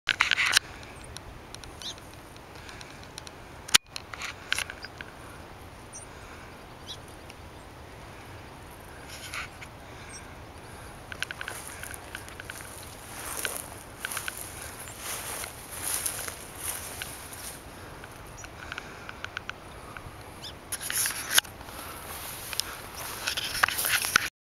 Elk at Fern Canyon Beach
Elk Grazing by Fern Canyon parking lot